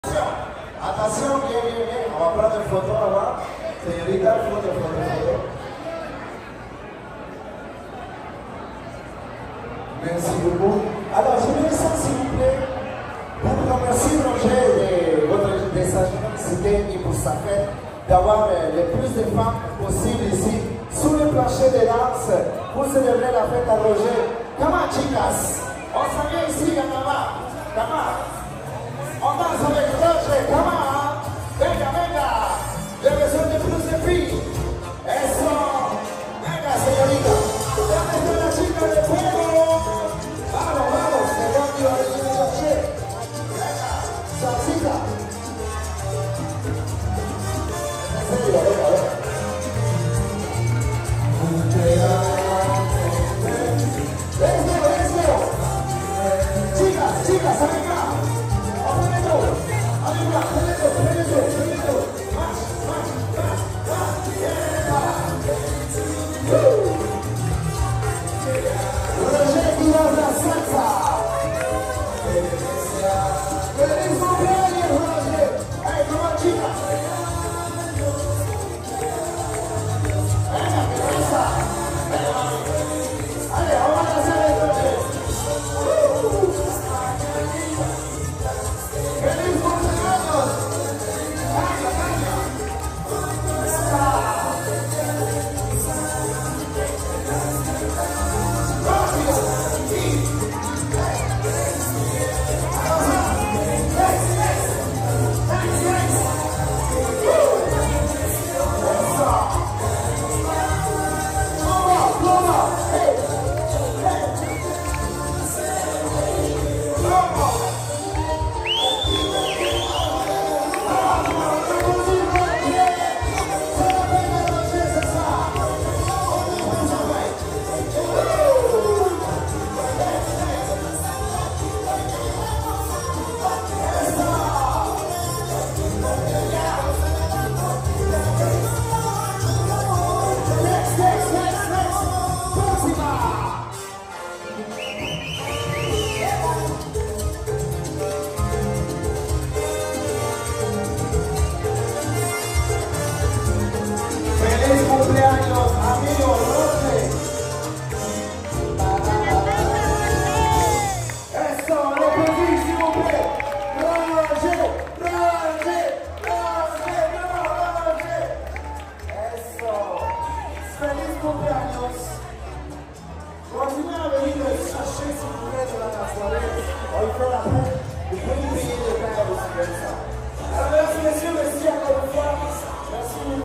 Fête à Roger (Salsa Birthday Dance) à la soirée Salsamania au Grand Studio le 7 juillet 2024
Laval, Canada